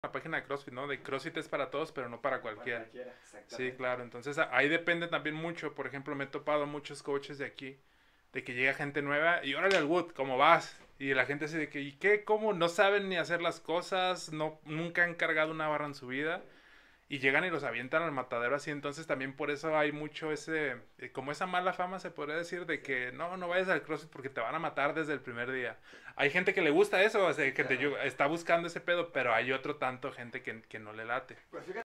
Como fue tu primer experiencia en crossfit?
Cuentanos como fue tu primer dia en una clase de crossfit? te preguntaron si antes habias practicado crossfit o algun otro deporte?